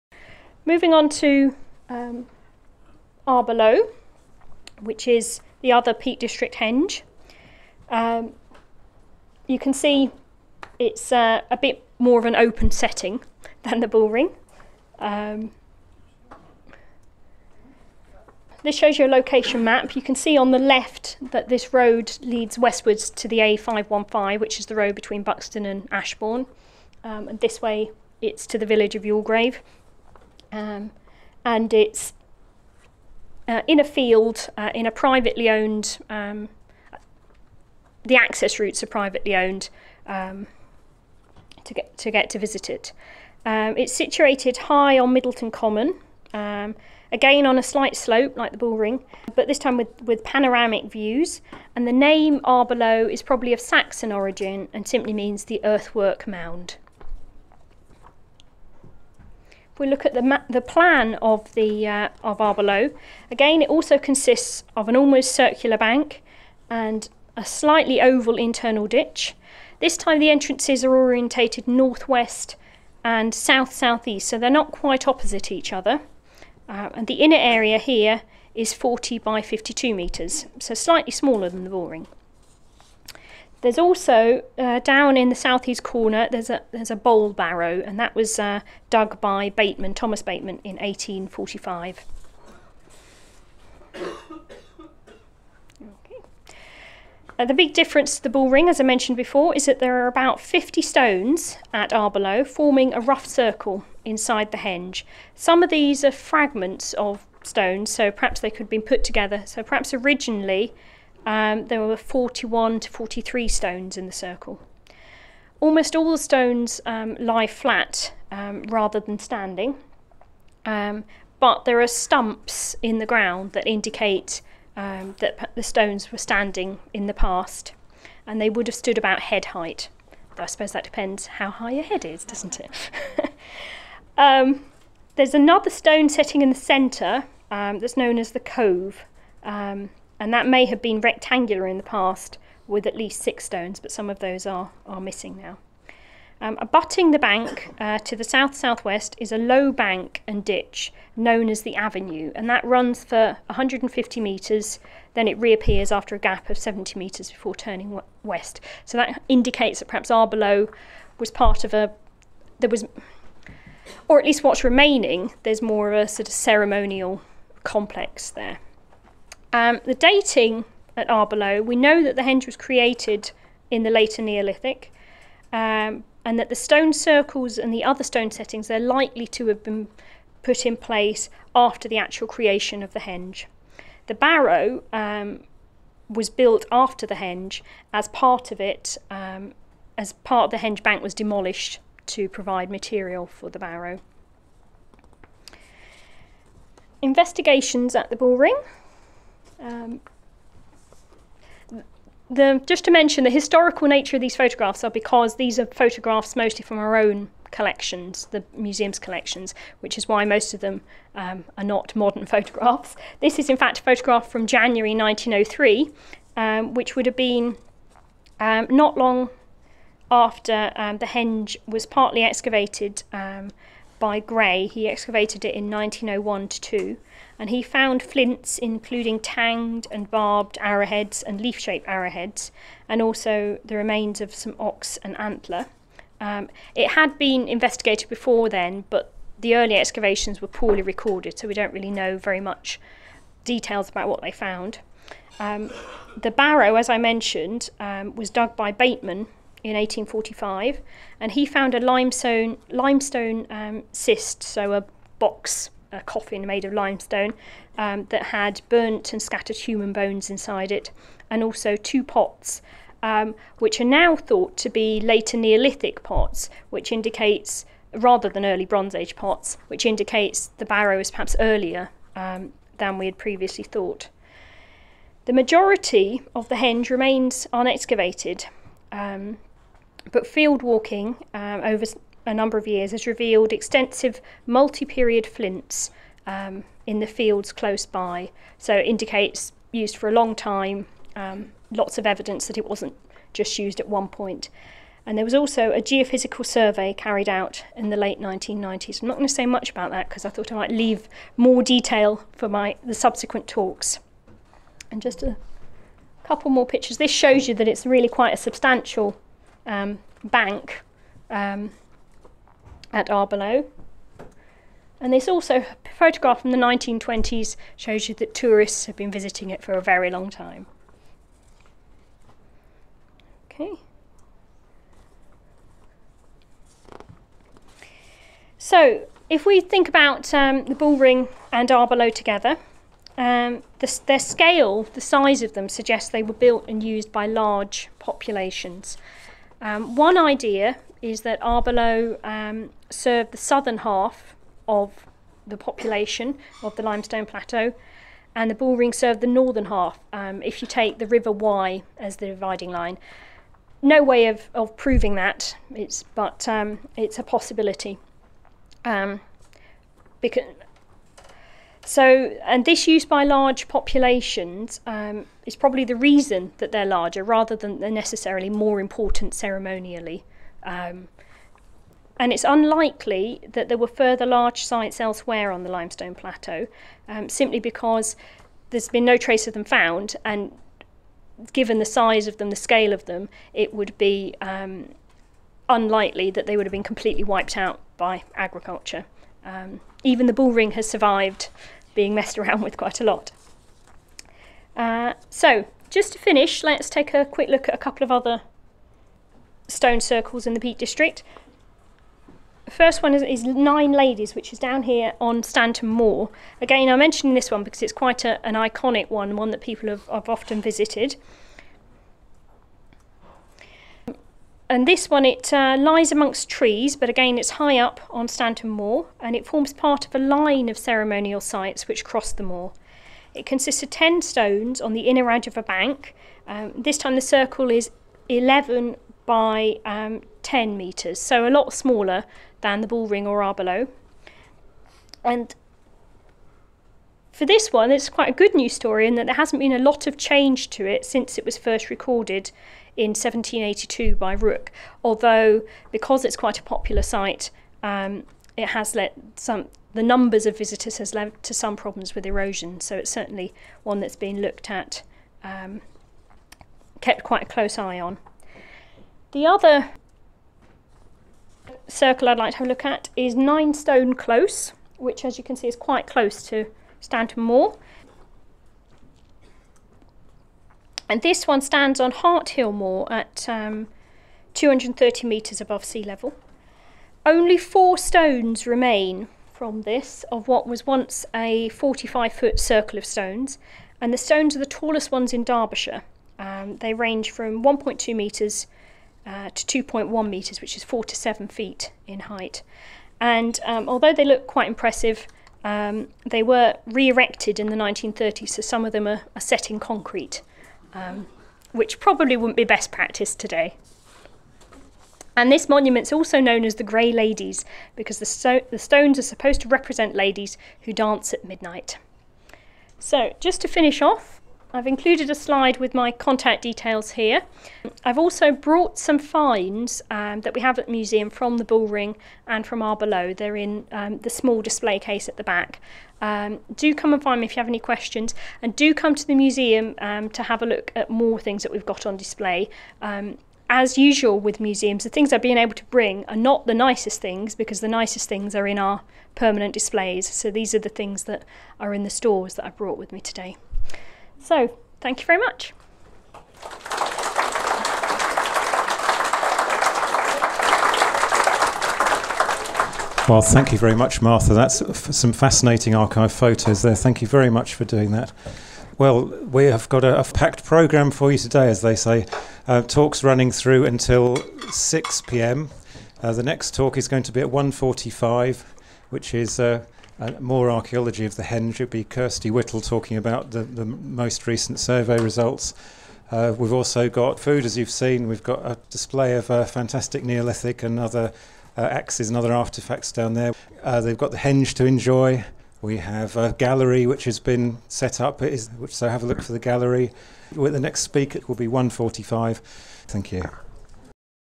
Martha Lawrence - The Bull Ring Henge and its closest relations Part 2 of 2
Martha Lawrence from Buxton Museum speaking at the Megalithic Portal's Bull Ring Open Day on Saturday 30th July 2011.